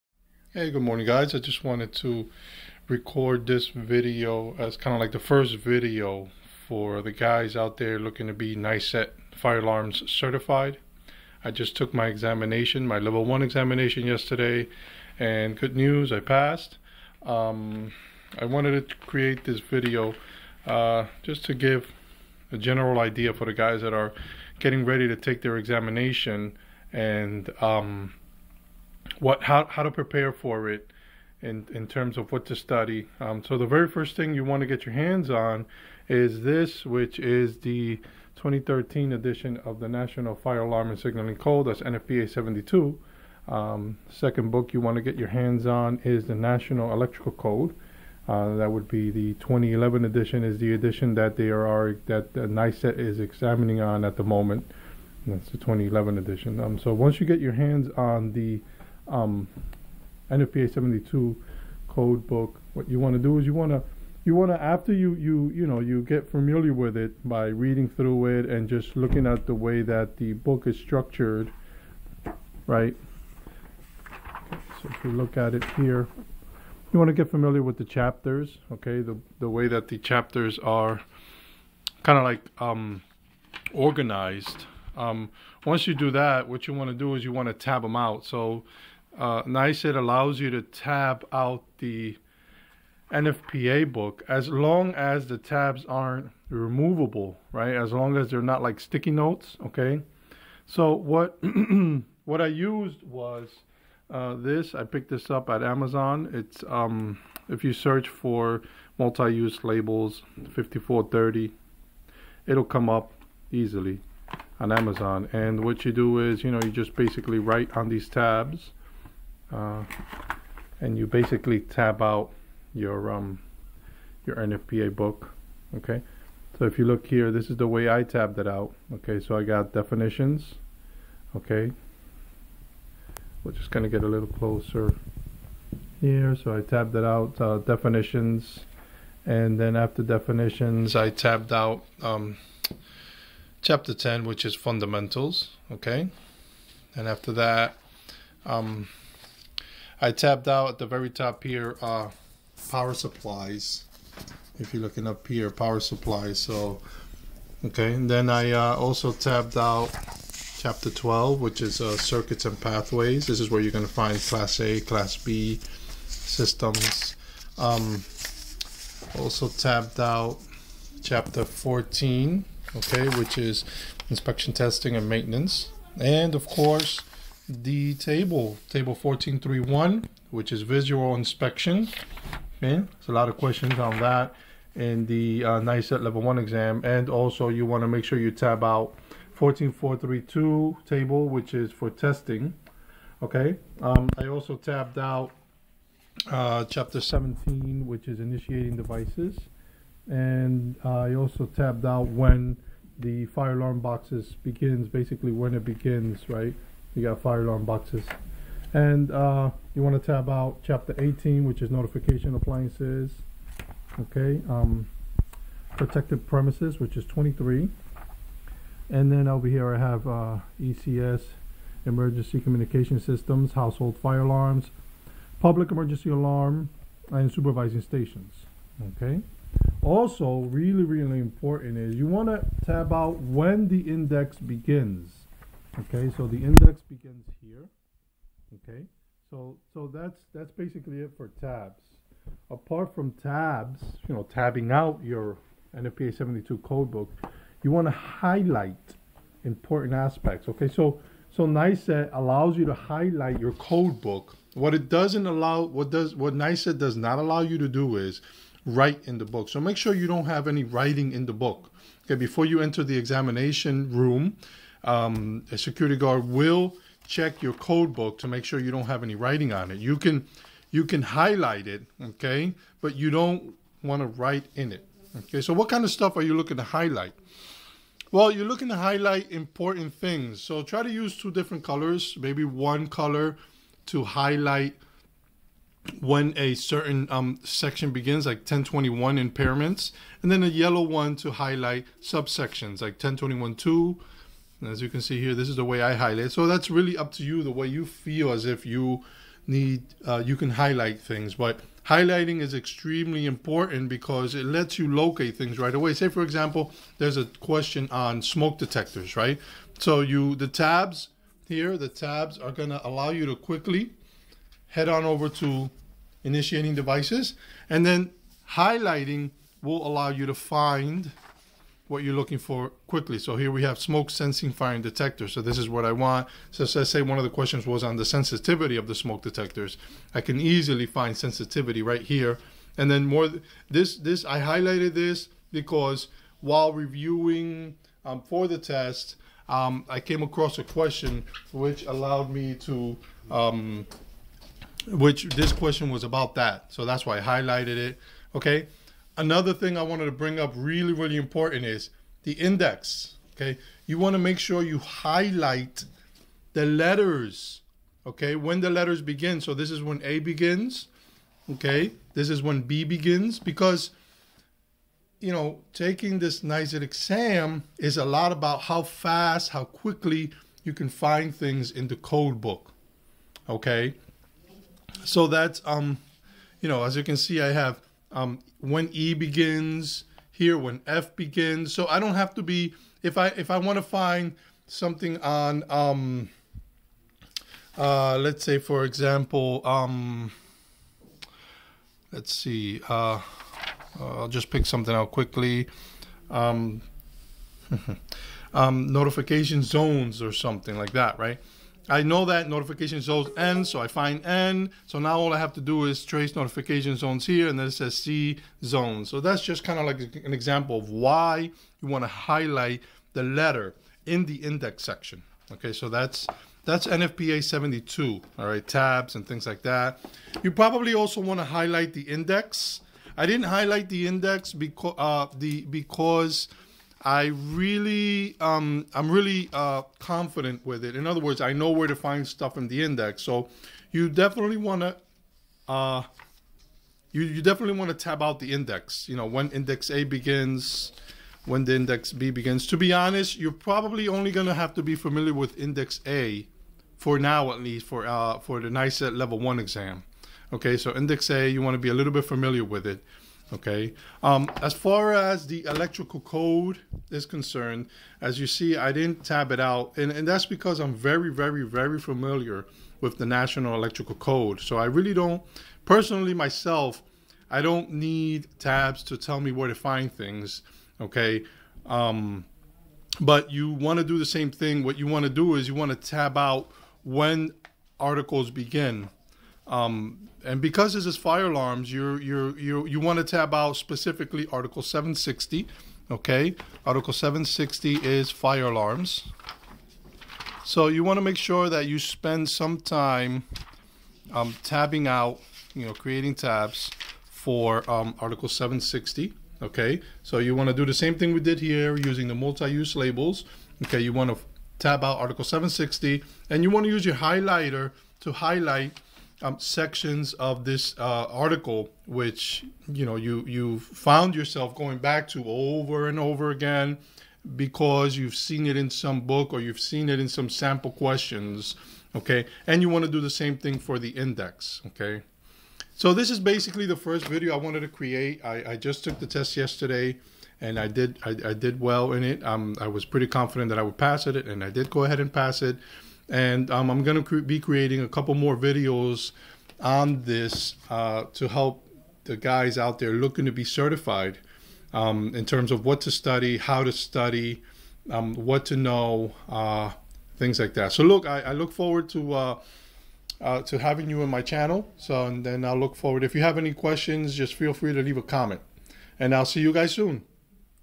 NICET Fire Alarms Level 1 Study Guide Intro
What you need to take the NICET Fire Alarms Level 1 exam and pass.  This is an introduction video explaining some of the books and resources I used to pass the exam.